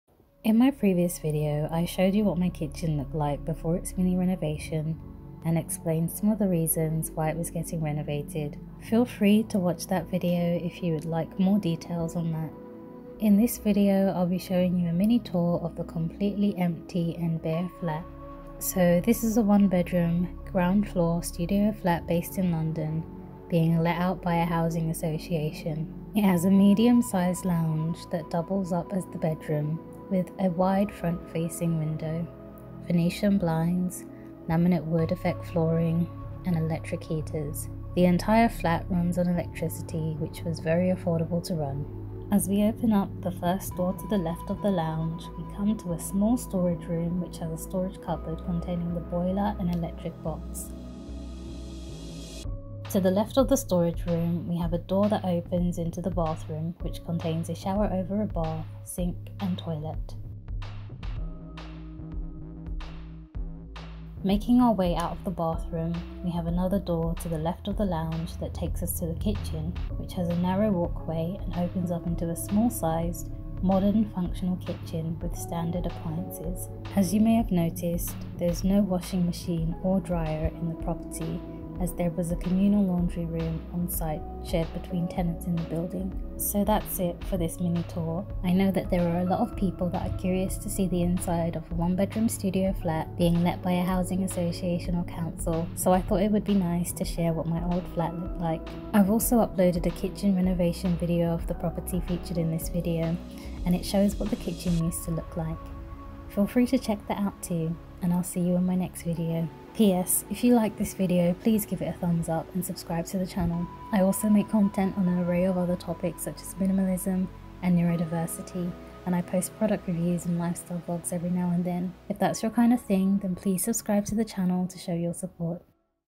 Housing Association/council housing studio in London | Empty flat tour
This video was recorded in May 2021 however I only just got around to uploading it. I have a few videos in this series which I thought would be good to share in order to give a realistic insight as to what a 1 bedroom, ground floor studio flat from a housing association can look like in UK, London.